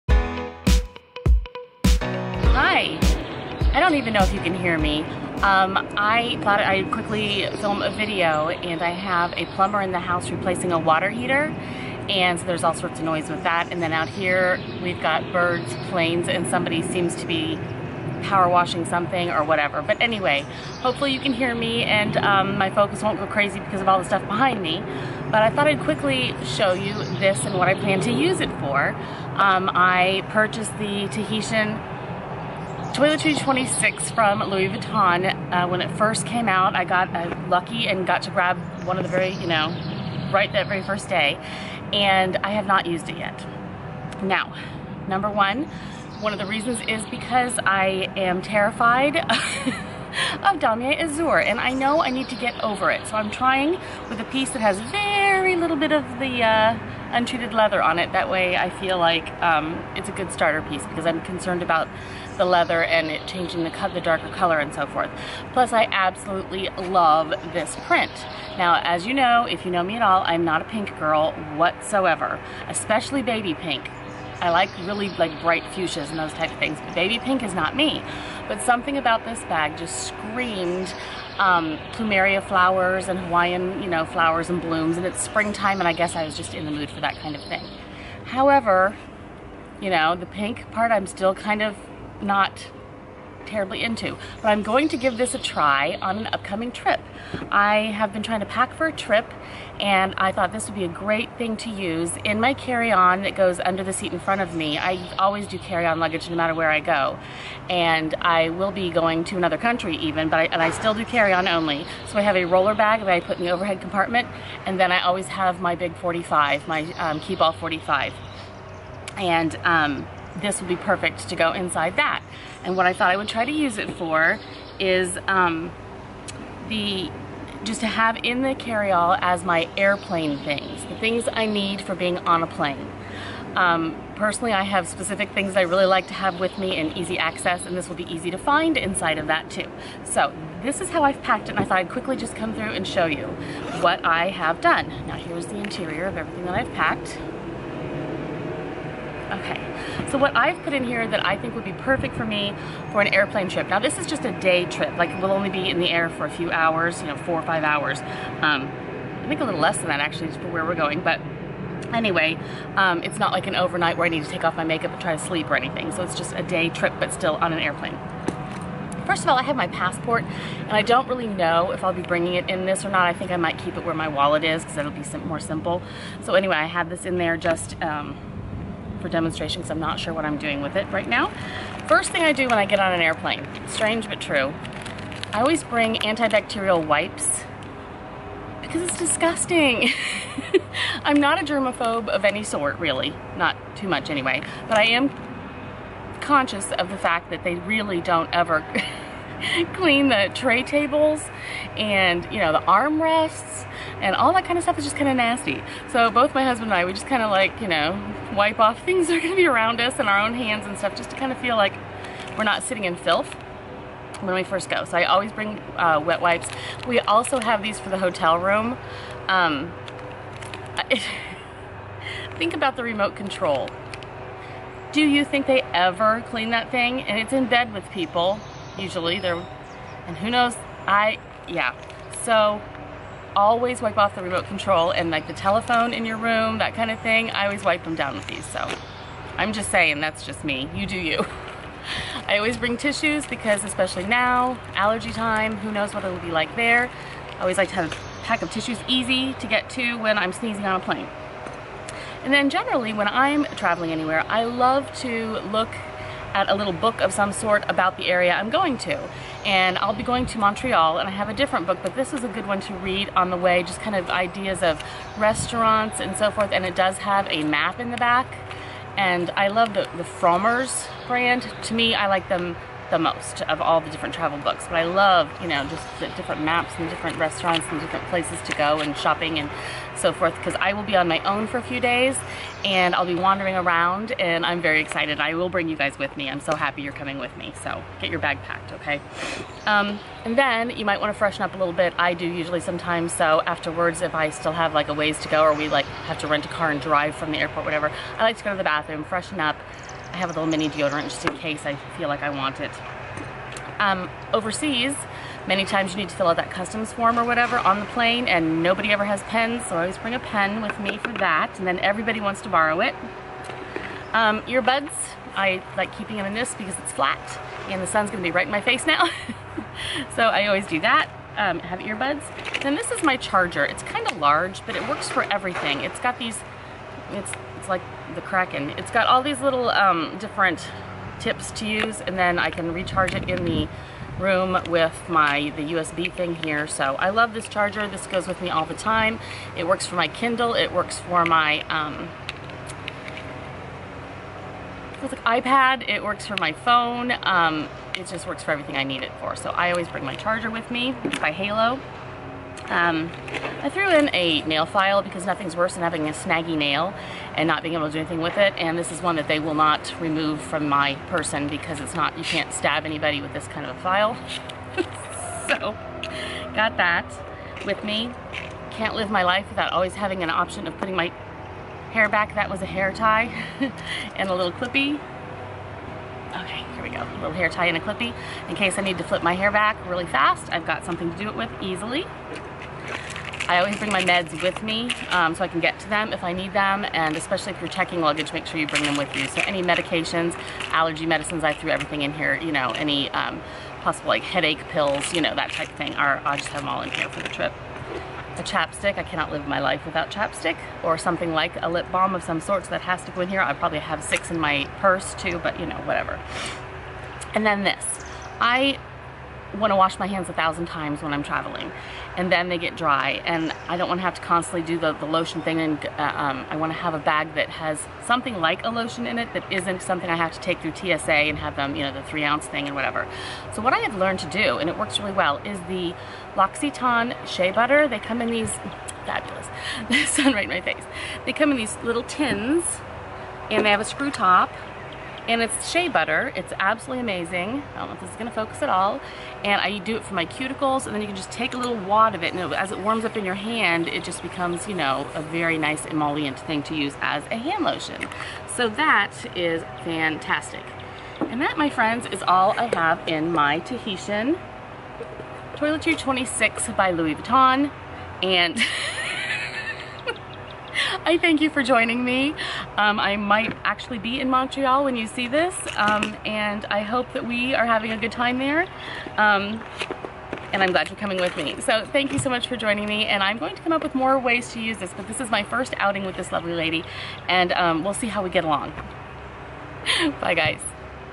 Louis Vuitton Toiletry 26 for Airplane Travel
I will be using my new Tahitian Toiletry 26 from Louis Vuitton on a trip to Montreal.  Here is how I have it packed for the flight.

Jennifer V.
U.S.A